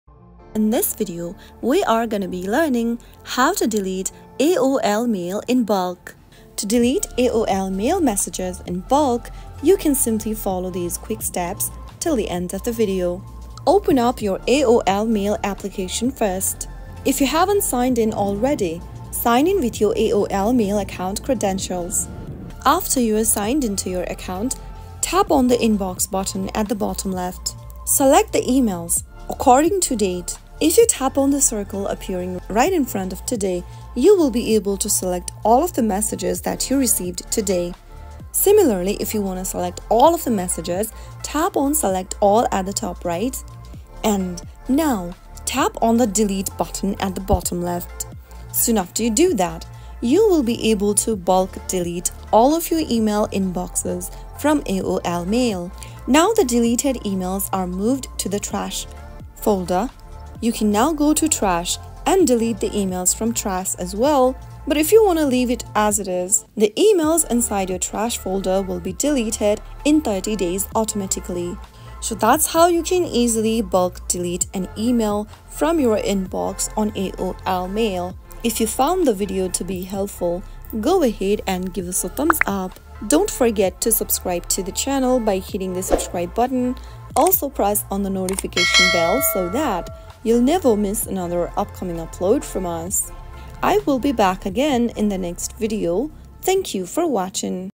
How to Delete AOL Mail in Bulk 2024?
In this tutorial, learn the efficient method to delete AOL Mail in bulk effortlessly. Whether you're looking to streamline your inbox or clear out unnecessary emails, this video provides a quick and easy solution to remove multiple emails from your AOL account at once. Discover the steps to efficiently manage your inbox and keep it organized without the hassle of deleting emails individually. Watch now and regain control over your AOL Mail with this handy bulk deletion technique.